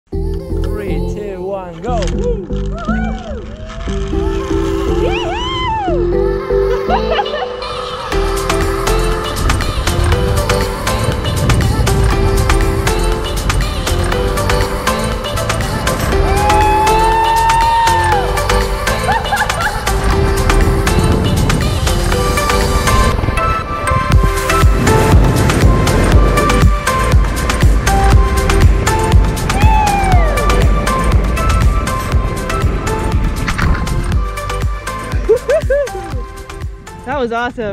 Visit Rapid City | Rushmore Tramway Adventures
Soar through the sky on the Pinnacle Zipline Tour at Rushmore Tramway Adventures! Stunning views of the Black Hills surround you as your race through and above the trees on this thrilling tour! Find out more about Rushmore Tramway Adventures